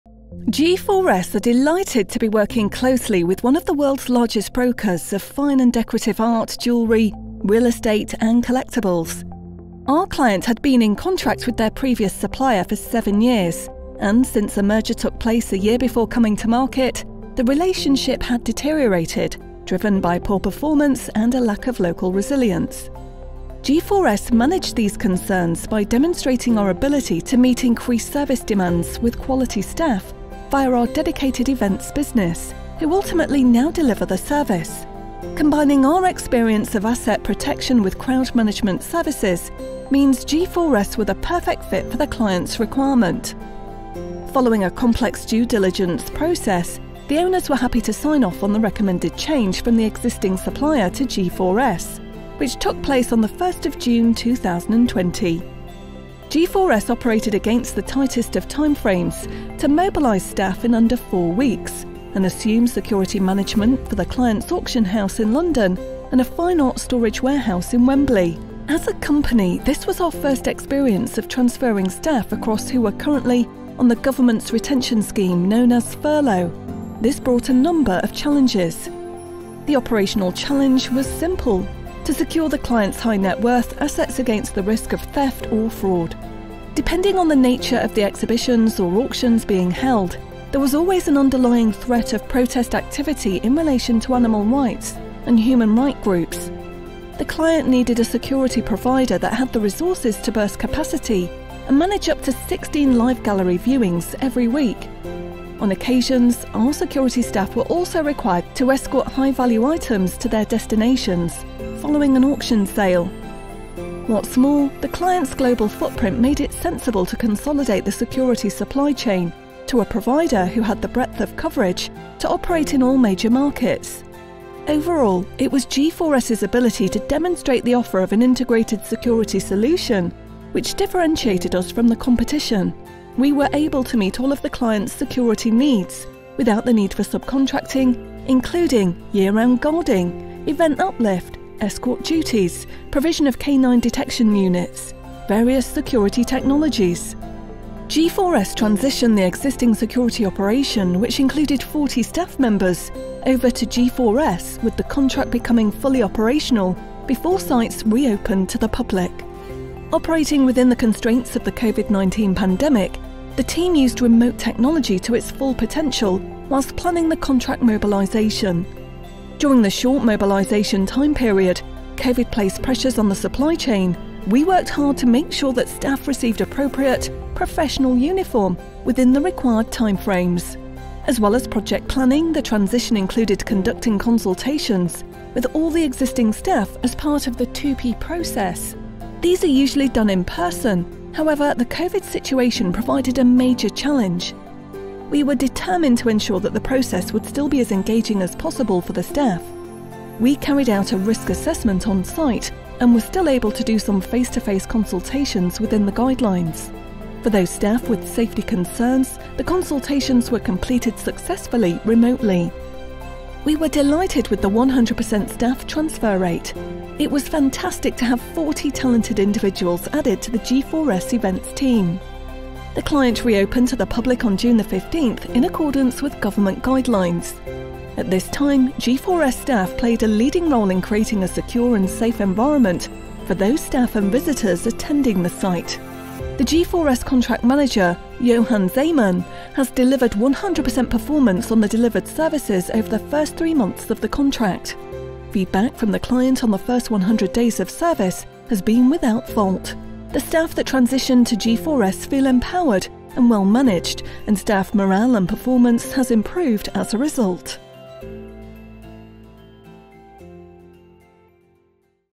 Mobilising a Contract at Short Notice
Find out how we undertook a successful contract mobilisation in short notice with TUPE transfer under lockdown.